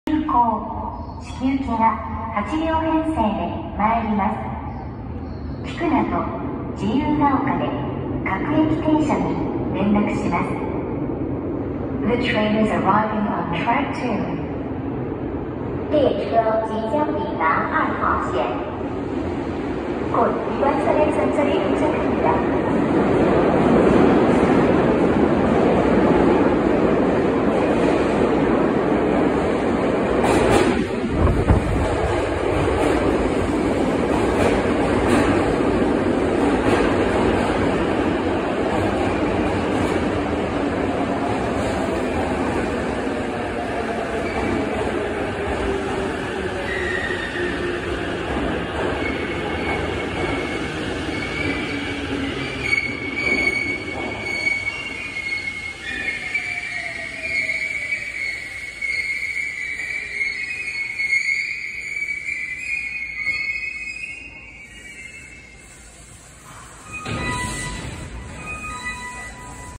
横浜高速鉄道みなとみらい線急行志木行き元町中華街駅入線シーン
東京メトロ7000系7034f充当でした。